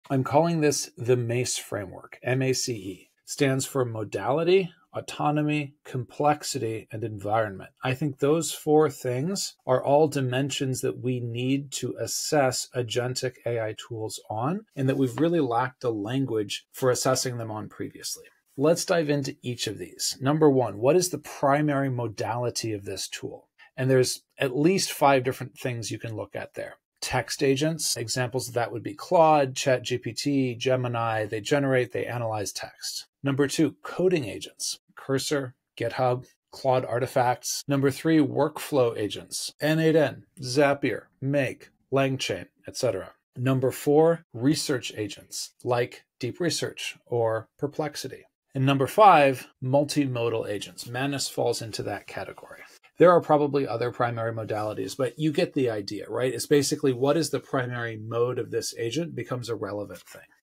AI Agents: The MACE Framework
Takeaways
 1. MACE Framework for Agents: Manus introduces a new way to assess AI agents—Modality, Autonomy, Complexity, and Environment—filling a gap in how we evaluate these tools.
 2. Six Agent Categories: Current AI agents fall into six buckets: conversational generators, coding assistants, workflow orchestrators, research synthesizers, autonomous executors, and hybrid collaboration tools.
 3. Manus as a Specialist Tool: Manus sits in the autonomous executor category, optimized for reliability and capability, but at a cost premium, making it more of a “surgeon’s scalpel” than a Swiss army knife.
 4. Enterprise Scaling Challenges: Multi-agent orchestration faces major hurdles—state management, memory, error handling, context control, and cost predictability—slowing enterprise adoption.
 5. Clear ROI Use Cases: Manus shines in high-value, specialist workflows such as research reports, content marketing pipelines, data analysis for non-technical teams, process documentation, and technical prototyping.
 6. Cost vs. Reliability Trade-off: Manus embodies the classic engineering dilemma—you can’t have reliability, capability, and low cost all at once; their choice is reliability + capability.
 7. Market Signal: Manus is a canary in the coal mine, previewing where multi-agent orchestration is headed and why major model makers will likely follow with similar offerings.

Quotes
“We need better language to describe agents, because otherwise we end up making inappropriate comparisons.”
“You can’t optimize for reliability, capability, and cost all at once—you have to pick two.”
“Manus is less a Swiss army knife and more a surgeon’s scalpel, designed for specialist tasks with clear ROI.”

Summary
In this talk, I introduce the MACE framework (Modality, Autonomy, Complexity, Environment) to better evaluate AI agents and map out six categories of tools in today’s landscape. Manus.ai, which launched in March 2025, has stabilized enough to warrant serious discussion. Positioned as an autonomous executor, Manus optimizes for reliability and capability at the cost of affordability, making it a specialist tool. Its strengths show up in use cases like research, content production, data analysis, and prototyping. While it faces scaling and cost challenges, Manus previews the next wave of enterprise-ready multi-agent orchestration.